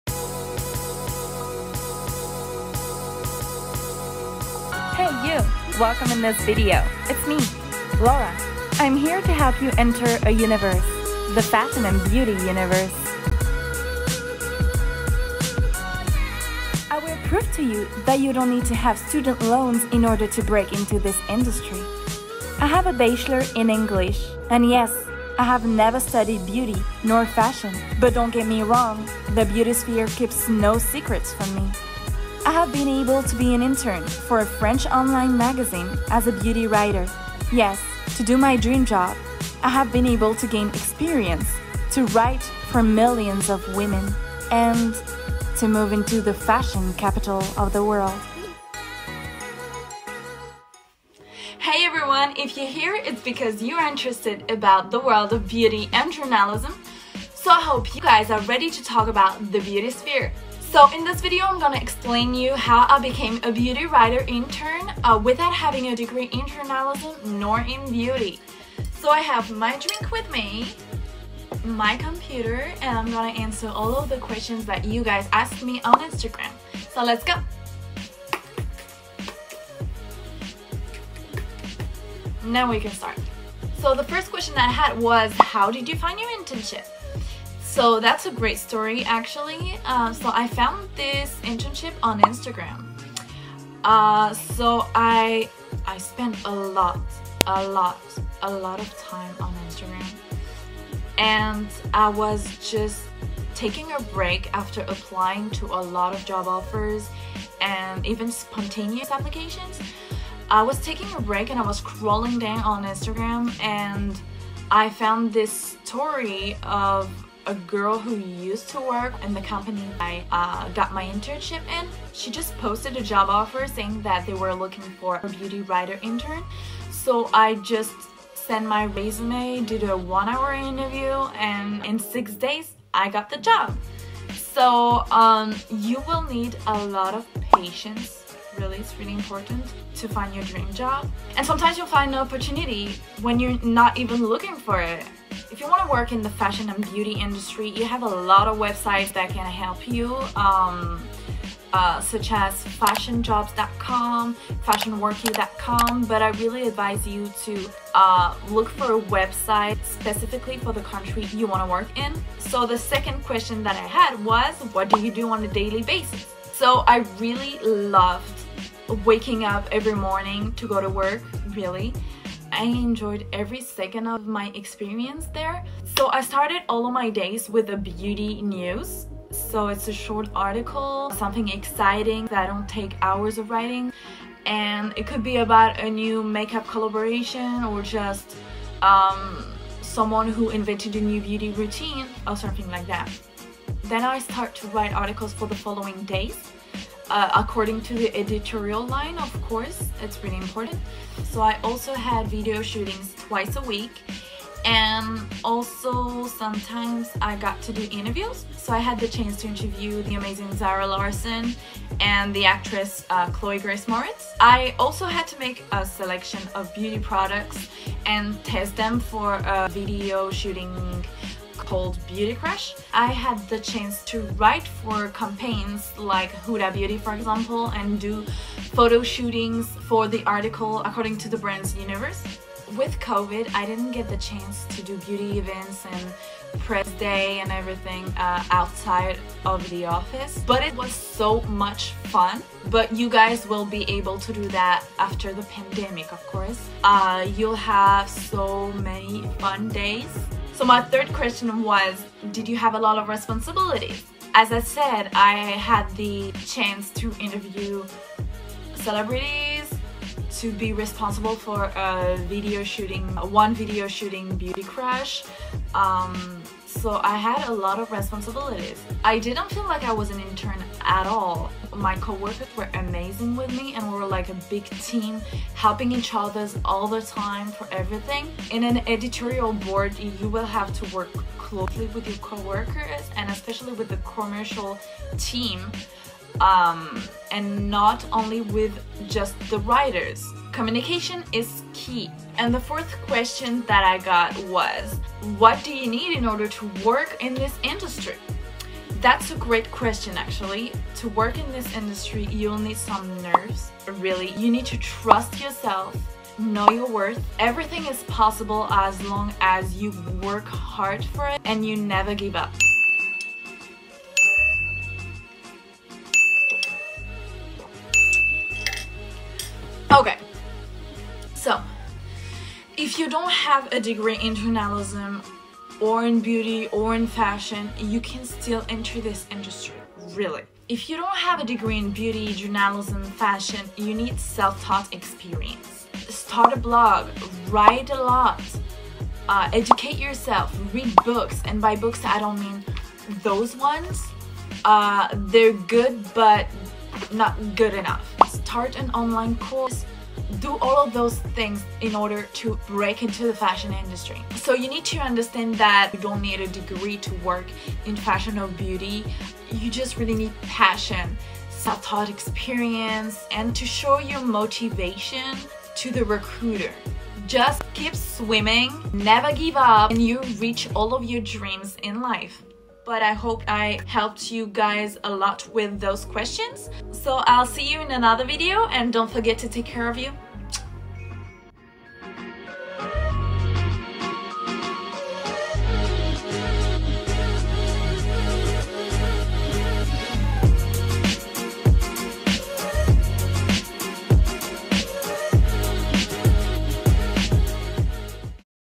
How To Become A Beauty/Fashion Writer Without A Degree
Have you ever dreamed about working in the fashion or beauty industry? It is possible without any degree! All you need is passion and to trust yourself. I did it, and you can too! If you have any questions I'll be glad to answer them.